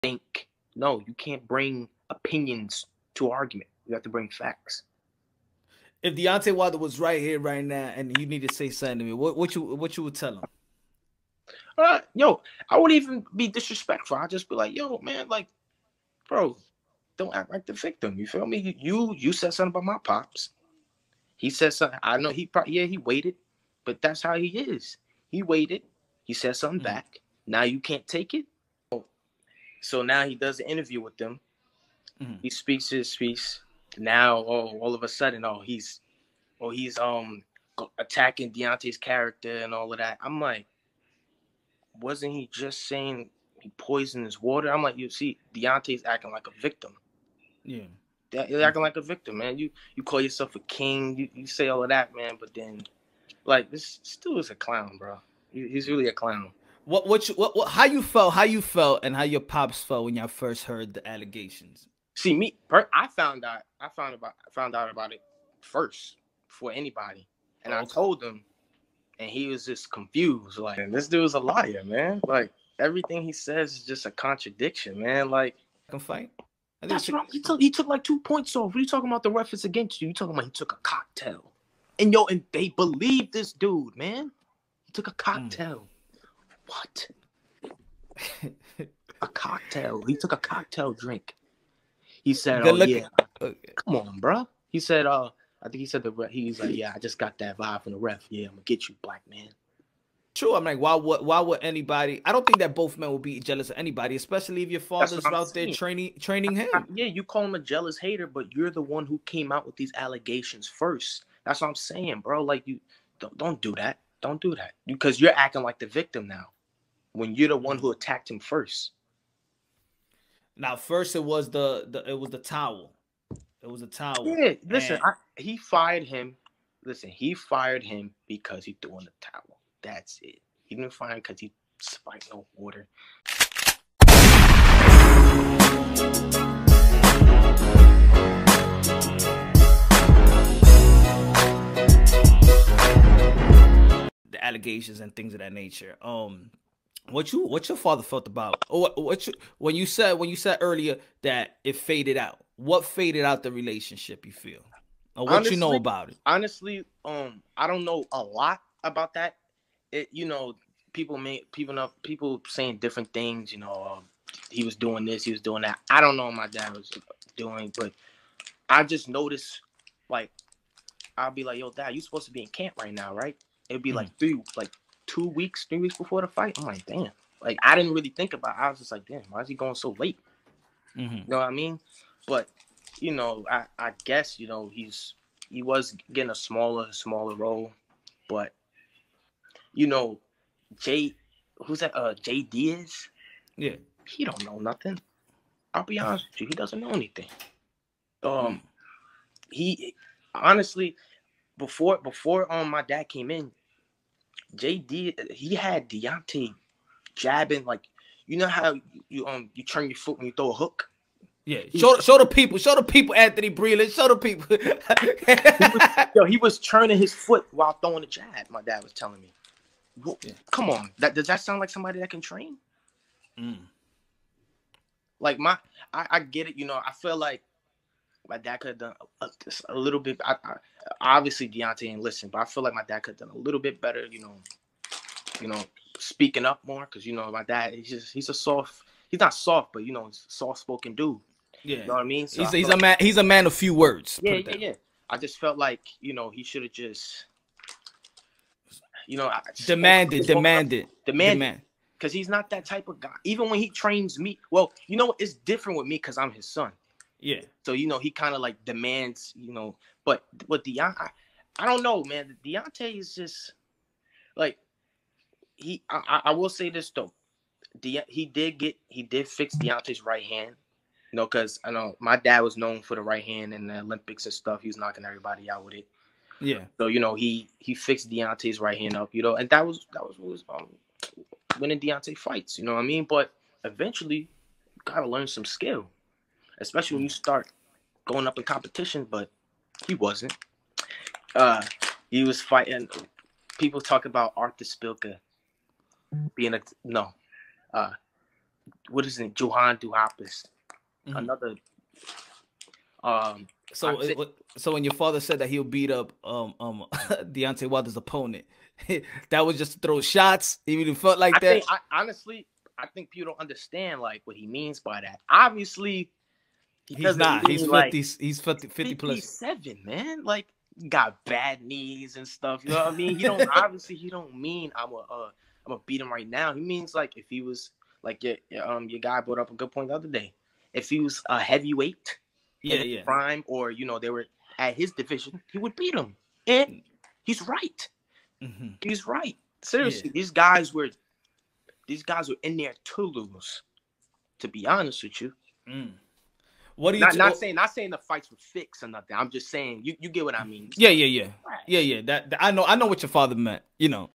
Deontay Wilder CHECKED by Mark Breland SON WOW!!! “I won’t let you tarnish my father’s LEGACY”
Mark Breland son Anthony Breland Speaks out against The false allegations and accusations from Deontay Wilder towards his Father.

FULL INTERVIEW VIDEO

youtu.be/f7pjQ4HNLpg

PODCAST  “THE SMELLING SALT”